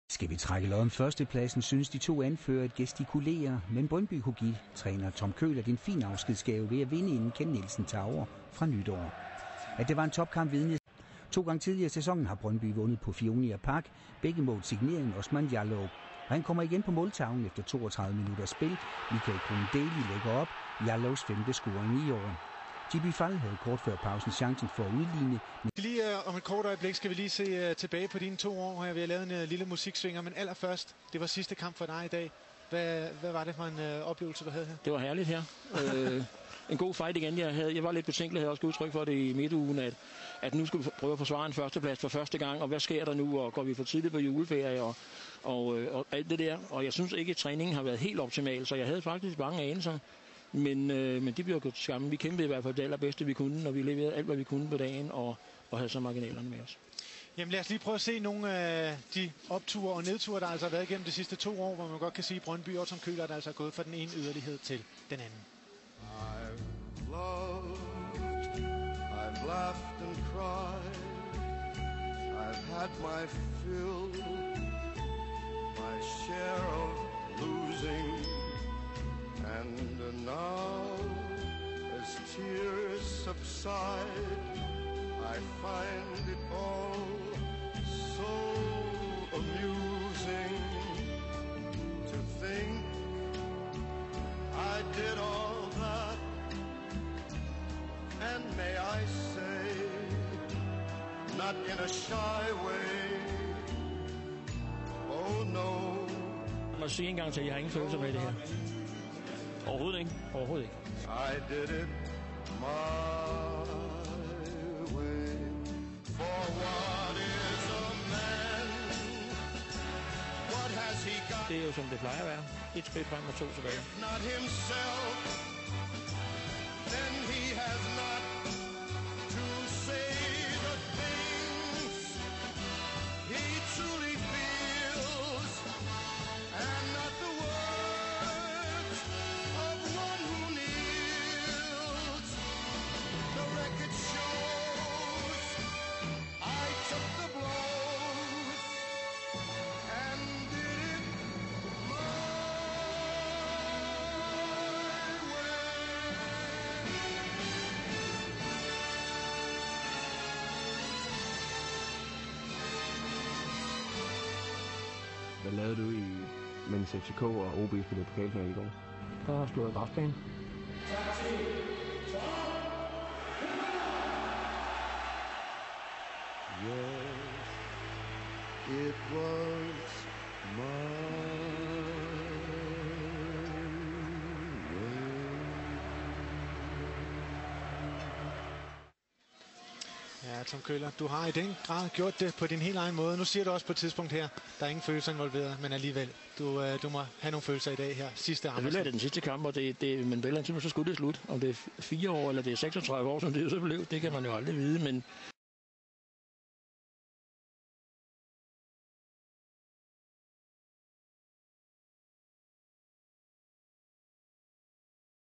Farvel Til Tom Køhlert
Farvel Til Tom Køhlert - Goddag Til En Forfærdelig Fremtid Med Kent Nielsen! Nu er tilværelsen dog blevet bedre med Henrik Jensen ved roret!